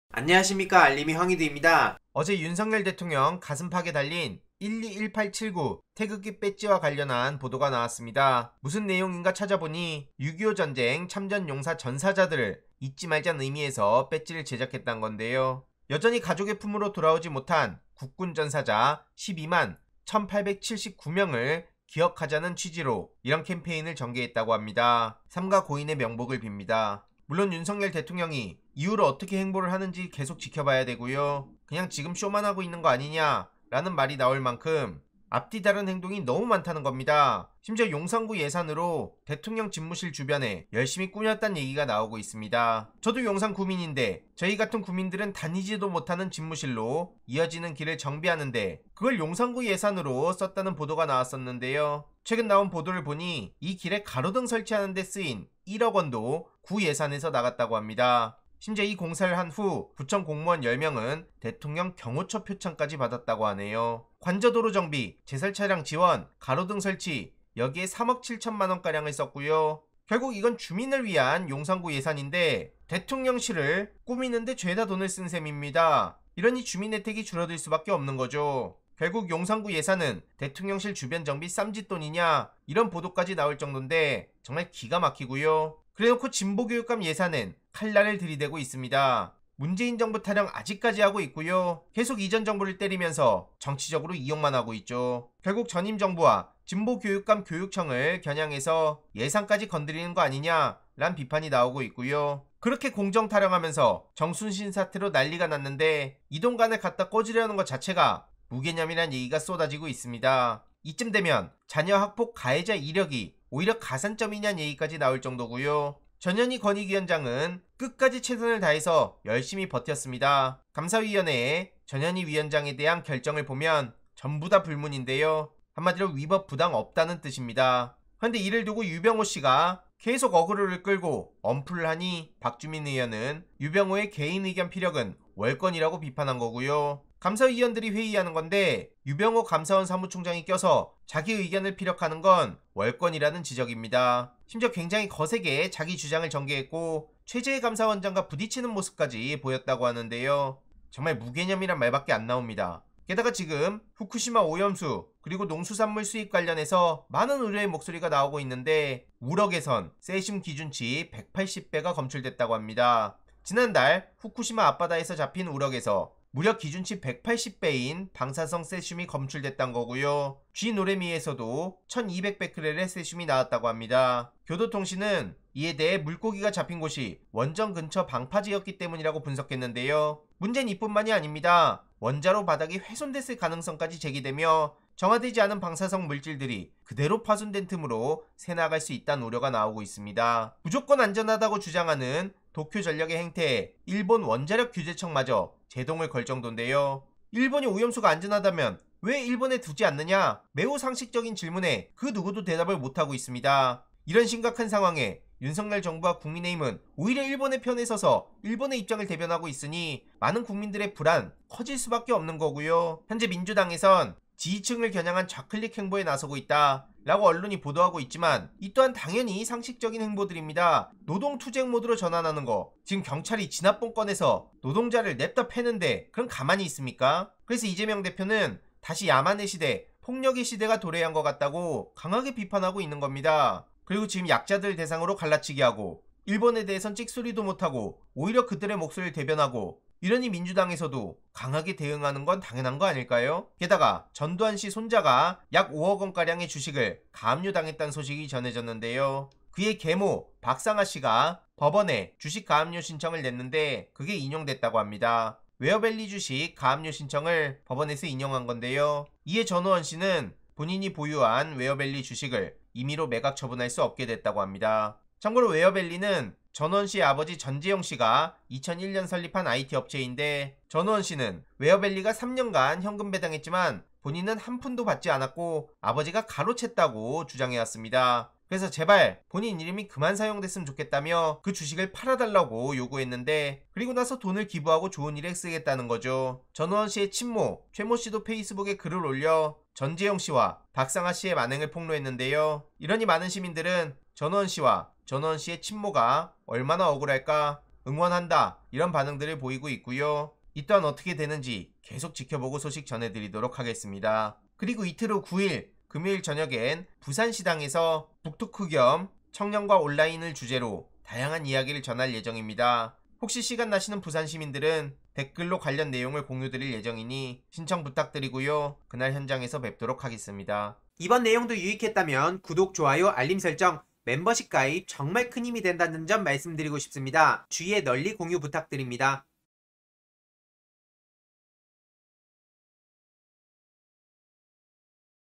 또 탄로난 윤석열 대통령실 만행! "용산구 예산으로 대통령 집무실 주변 가로등에 1억" - 우럭 세슘 기준치 180배, 원자로 손상 논란.. 국민 불안 외면하는 국민의힘
영상 설명 : 또 탄로난 윤석열 대통령실 만행, 용산구 예산으로 대통령 집무실 주변 가로등에 1억, 우럭 세슘 기준치 180배, 원자로 손상 논란, 국민 불안 외면하는 윤석열 대통령과 국민의힘 소식 등을 요약해봤습니다.

尹 가슴팍에 달린 '121879 태극기' 배지…의미 알고보니

윤석열 대통령이 6일 현충일 추념식에 참석하며 양복 가슴팍에 단 배지가 눈길을 끌었다. 배지는 태극기 문양에 '121879' 숫자가 적혀 있다.

이는 국가보훈부가 올해 6·25전쟁 정전 70주년을 맞아 '끝까지 찾아야 할 121879 태극기' 대국민 캠페인을 진행하며 제작한 배지다. 지난 2020년 광운대 공공소통연구소가 6·25전쟁 참전 용사 전사자들을 잊지 말자는 의미에서 제작한 배지에서 착안한 것이다.

앞서 국가보훈부는 여전히 가족의 품으로 돌아오지 못한 국군 전사자 12만1879명을 기억하자는 의미에서 이 같은 캠페인을 전개한다고 밝혔다.

배지는 6·25전쟁 참전 용사 전사자를 모신 함에 도포된 태극기 형상에 착안해 만들어졌다. 6·25전쟁 참전 용사에 대한 국가의 역할과 책임을 표명함과 동시에 국민과의 굳건한 약속을 다짐하는 보훈의 참 의미를 담았다고 보훈부는 설명했다.

한편 윤 대통령은 이날 추념사에서 "대한민국을 지키기 위해 우리 국군 16만명이 전사했지만, 12만명의 유해를 찾지 못했다. 동맹국인 미군도 3만7000명이 전사했다”며 “정부는 호국 영웅들께서 가족의 품으로 돌아갈 수 있도록 끝까지 노력하겠다"고 약속했다.

[단독] 용산구 예산으로 대통령 집무실 주변 '가로등에 1억'

[앵커]
현 정부가 대통령 집무실을 이사하며 쓴 예산이, 공개된 것 말고도 곳곳에 숨어있단 지적이 있었죠. 저희 JTBC도 정작 용산구민들은 다니지 못하는, 집무실로 이어지는 길을 정비하는 비용도 용산구 예산에서 나갔다고 보도해 드렸는데요. 더 취재를 해보니, 이 길에 가로등을 설치하는 데 쓰인 1억 원도 구 예산에서 나간 걸로 확인됐습니다. 이 공사를 한 뒤 구청 공무원 10명은 대통령 경호처 표창도 받았습니다.

김안수 기자가 취재했습니다.

[기자]

높은 담벼락이 있습니다.

위에는 철조망도 쳐져 있습니다.
서울 이촌역 2번 출구 바로 앞 입니다.

이 뒤로 쭉 가면 대통령 집무실이 나옵니다.

주변 도로는 경호 때문에 모두 통제돼 있습니다.

용산구민을 포함해 일반시민은 이용할 수 없습니다.
그런데 이 길에 용산구청이 1억 천 7백만원을 들여 가로등 22개를 설치한 사실이 취재결과 확인됐습니다.

지난해 5월 국토교통부가 용산구청에 공문을 보내 도로시설물 정비를 요구했기 때문입니다.

이미 집무실 주변 도로 등을 보수하느라 2억 6천만원 쓴 사실도 JTBC 보도로 드러났습니다.

정순신 겪고도 이동관?... "윤 대통령, '자녀 학폭 이력'에 가산점 주나"

이동관 대통령실 대외협력 특별보좌관이 방송통신위원장에 사실상 내정된 것으로 알려지면서, 이 특보 아들의 과거 학교폭력 사건이 다시 수면 위로 떠오르고 있다.  더불어민주당은 "'자녀 학폭 가해자 이력'이 윤석열 정권에선 공직 임명의 가산점이냐"며 맹공을 퍼부었다.
강선우 민주당 대변인은 6일 국회 소통관 브리핑을 통해 "이명박 정부 때인 지난 2011년, 이동관 특보 아들의 학폭 사건 은폐 의혹이 제기됐다"라며 "정순신 국가수사본부장이 아들 학폭 문제로 사퇴한 게 불과 석 달 전이다, '자녀 학폭 가해자 이력'이 윤석열 정권에선 공직 임명의 가산점이냐"라고 힐난했다.

강 대변인은 "자녀 학폭 가해 사실이 있어도 한 자리 해 먹어보겠다는 뻔뻔한 결기 정도는 있어야 언론 장악도 하고 경찰 장악도 할 수 있다는 거냐"고 덧붙였다.

앞서 오마이뉴스는 이 특보 아들이 자율형사립고인 서울 하나고에 다니다 학교폭력 가해를 했지만 해당 학교는 법에 정해진 학교폭력대책자치위 조차 열지 않고 전학으로 사건을 무마한 의혹을 보도했다. (관련 기사 : "침대에 눕혀서 밟았다"...'학폭' 이동관 아들, 학폭위 없이 전학 명문대 진학)

강 대변인은 "이동관 특보는 이명박 정부 청와대 시절 정권에 부정적인 보도들에는 고소 남발로 언론에 재갈을 물렸고, 공영방송 사장 해임, 낙하산 사장 선임, 언론인 해직 사태 등을 주도하며 언론장악 최선봉에 섰던 인물"이라며 "공정성과 중립성을 훼손했다며 임기를 불과 두 달 남겨둔 한상혁 방통위원장을 면직한 윤석열 대통령이 임명하려는 자가 이동관 특보"라고 날을 세웠다.

강 대변인은 "공정성과 중립성이라곤 눈을 씻고 찾아봐도 찾을 수 없는 인사를 앉혀 이번에는 대한민국을 후진국 대열로 추락시키는 선봉장에 세우려는거냐"라며 "이것이 윤석열식 공정이냐, 내로남불의 전형"이라고 꼬집었다.

이어 "방통위법은 정파성을 배제하기 위해 '최근 3년 내 대통령직 인수위원'을 방통위원의 결격사유로 두고 있는데, '대통령 특보'는 인수위원이 아니기 때문에 괜찮다는 거냐"라며 "대통령 특보도 (방통위원) 결격사유로 명시하는 '이동관 방지법'이 필요할 지경"이라고 비판했다.

마지막으로 강 대변인은 "정치적, 법적, 도덕적 그 어떤 잣대로 봐도 이동관 특보는 방통위원장으로서 자격미달, 함량미달, 수준미달"이라며 "윤석열 대통령은 학폭 가해 책임이 있음에도 한 자리 해 먹겠다는 '무서운 결기'를 가진 이동관 특보 임명으로 방송과 언론을 장악하려는 무도한 시도를 즉각 중단하라, 정말 답이 없는 정권"이라고 목소리 높였다.

전두환 손자 4.8억 주식, 가압류 당해…계모 누구?

전두환 전 대통령의 손자 전우원(27) 씨를 상대로 계모 박상아(51) 씨가 법원에 낸 주식 가압류 신청이 인용됐습니다.
6일 법조계에 따르면 서울서부지법 민사51단독(박인식 부장판사)은 박씨가 지난달 10일 우원씨를 상대로 낸 약 4억8232만원 규모의 웨어밸리 주식 가압류 신청을 같은 달 17일 인용했습니다.

이에 따라 우원씨는 자신이 보유한 웨어밸리 주식을 임의로 매각·처분할 수 없게 됐습니다.

본안 소송이 제기된다면 이 지분을 두고 모자 간 법정 다툼을 벌일 가능성도 있습니다.

웨어밸리는 전 전 대통령 차남이자 우원씨의 아버지 전재용씨가 2001년 설립한 IT업체입니다. 2013년 검찰 ‘전두환 일가 미납 추징금’ 특별환수팀은 비자금 관리인이자 웨어밸리 대표 손삼수씨로부터 전 전 대통령의 비자금 5억5000만원을 환수하기도 했습니다.

당시 손 씨는 웨어밸리 최대주주(지분율 49.53%)였고 재용 씨의 두 아들인 전 씨와 전우성 씨도 각각 7%의 지분을 보유했습니다.

용산구 예산은 대통령실 주변 정비 쌈짓돈?…가로등 설치에 1억 쓰고 못 돌려받아

"노동자 두들겨 팬다" 거리 나선 친명·비명…野 '좌클릭' 노림수

최근 민주당은 ‘윤석열 정부 건설노동자 탄압 및 과잉수사 대응 태스크포스(TF)’를 구성하는 등 노동문제에 적극적으로 나서고 있다. 특히 지난달 초 경찰 수사에 반발한 건설노조 간부가 분신한 데 이어 31일 포스코 광양제철소 앞에서 고공농성을 벌이던 한국노총 간부가 경찰의 진압봉에 맞아 부상 당한 뒤로 당내 기구의 활동이 눈에 띄게 잦아졌다. 지난해 말 정부가 화물연대 파업을 강경 대응하고 민주노총을 압수수색할 때만 해도 민주당은 공식 논평을 자제하며 ‘로우키(low-key)’로 대응했다. 당시 노조 대응에 대한 긍정적 여론이 윤석열 대통령 지지율 상승세를 이끌자 “노조 편을 들자니 여론이 부담된다”는 당내 반응이 적지 않았다.
그랬던 민주당은 이달 들어 다른 모습을 보이고 있다.

이재명 대표는 연일 정부의 노조 진압에 대해 “다시 야만의 시대, 폭력의 시대가 도래한 것 같다”며 목소리를 높이고 있다. TF 단장인 진성준 의원이 지난 4일 기자간담회에서 “양대 노총의 장외투쟁에 당이 적극적으로 결합해 함께 투쟁하는 문제도 검토하자”고 제안한 데 이어 7일엔 당내 노동존중실천단(단장 서영교 의원)이 광양제철소를 방문해 한노총과 연대 투쟁에 나설 예정이다.

윤석열 정부, 진보 교육감 예산에 칼날…교부금 손질로 이어지나

우럭에서 세슘 기준치 180배‥'원자로 손상'도 논란

이와 관련한 내용들을 핵심만 간략하게 요약해봤습니다.